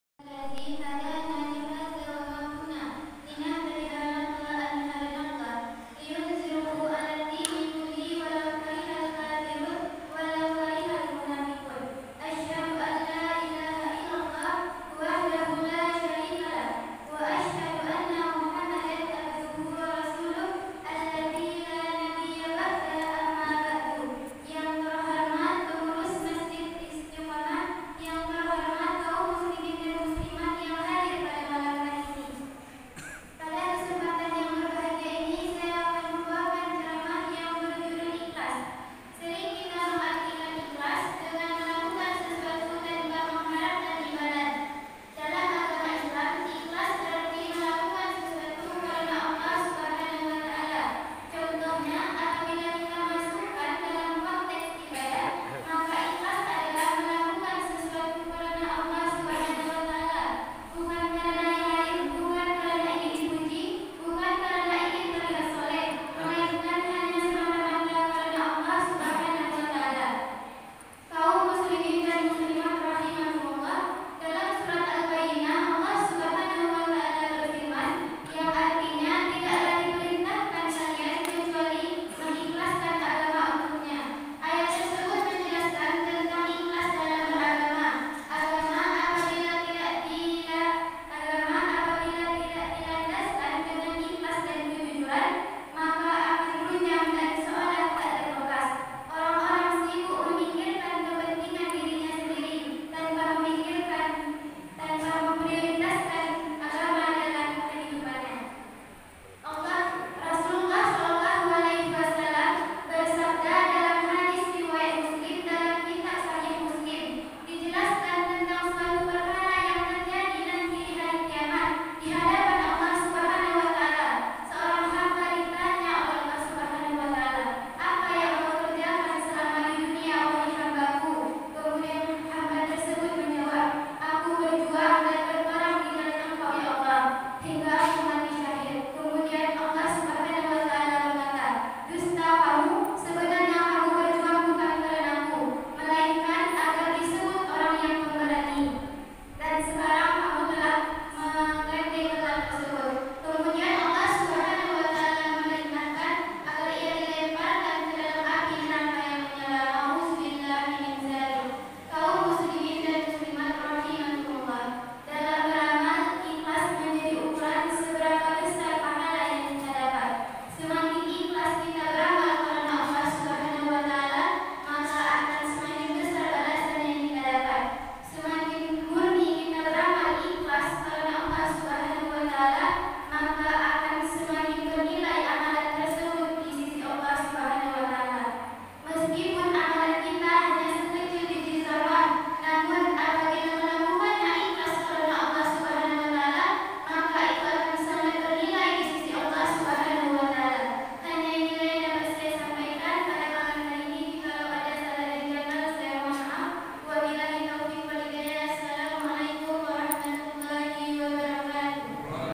kultum malam 8 ramadhan di masjid Istiqomah kota lama Rokan hulu ustadzah kecil Wina Handayani.
kultummalam8ramadhan#ustadhahkecil#masjidistiqomah